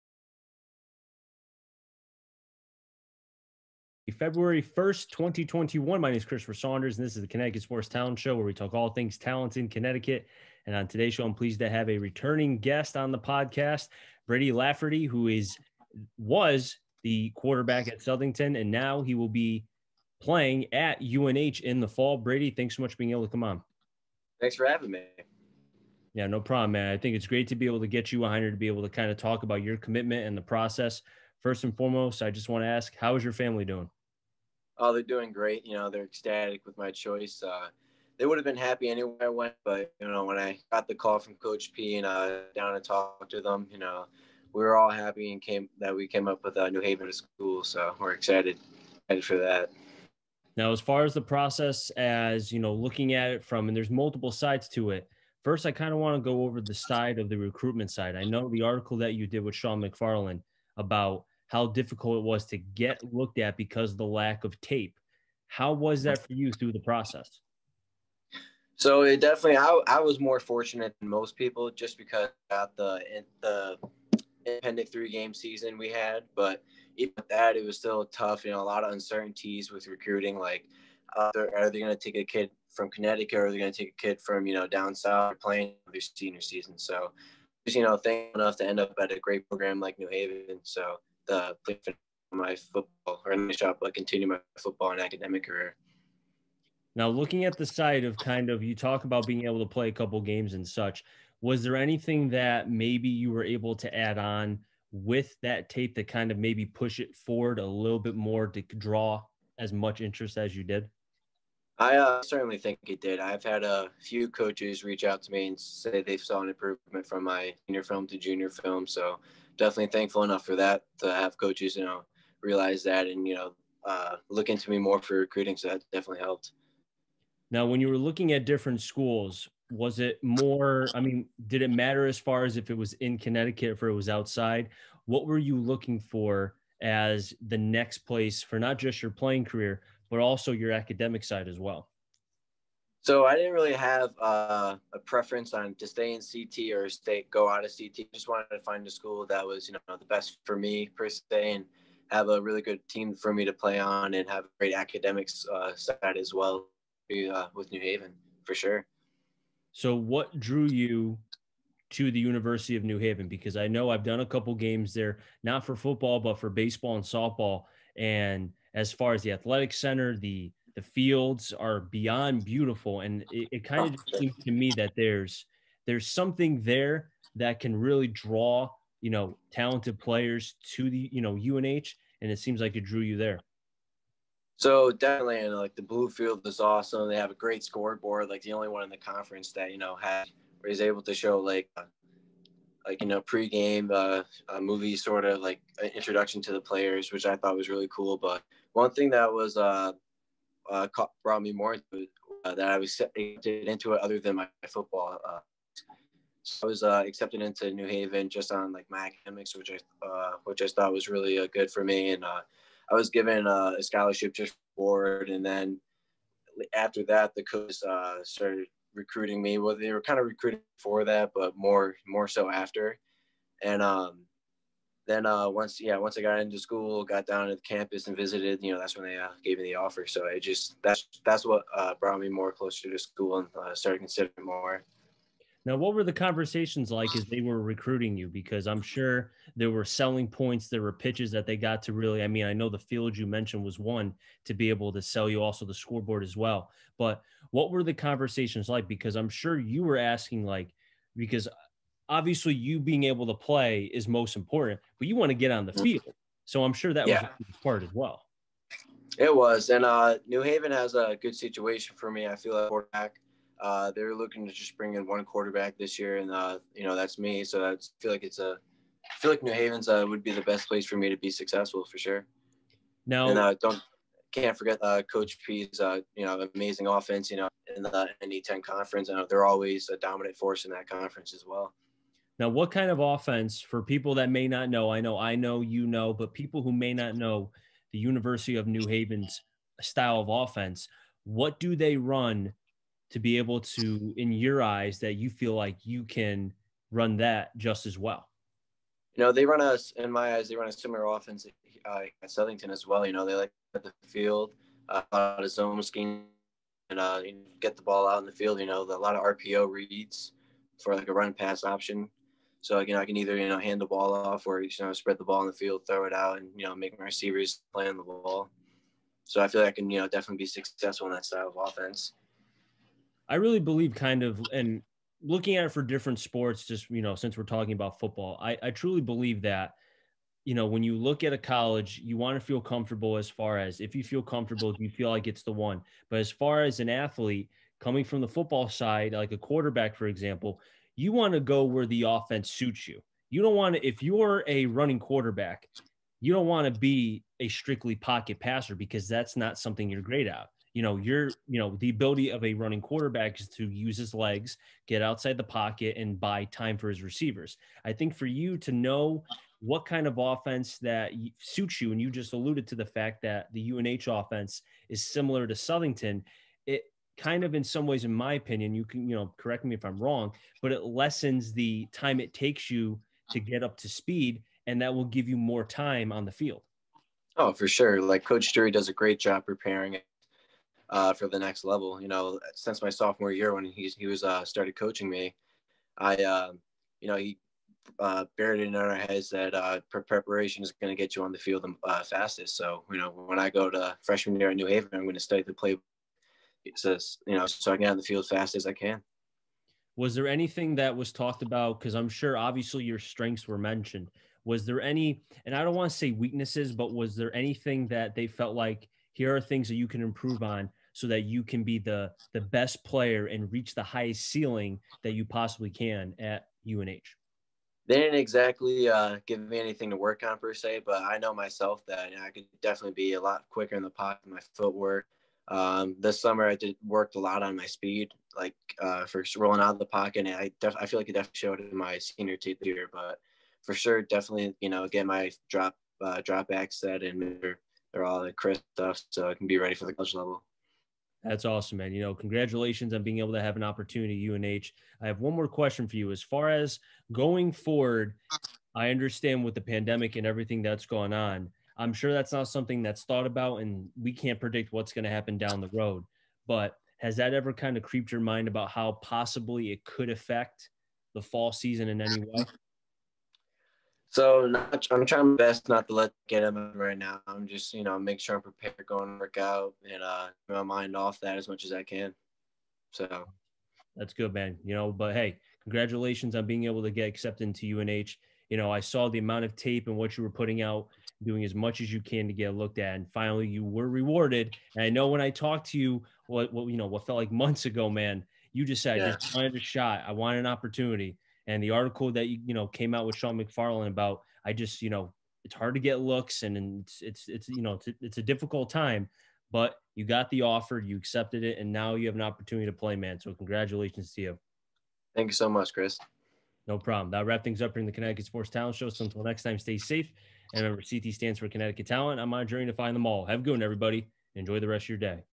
CT Sports Talent Show: Brady Lafferty HD 720p
The former Southington QB and now In-Coming Freshman at University of New Haven commit Brady Lafferty has worked his tail off to get to this spot in his career, and through covid that didn't stop him.

Brady is a returning guest, and I just love the makeup of this young man. We talk about his commitment and why he choose UNH.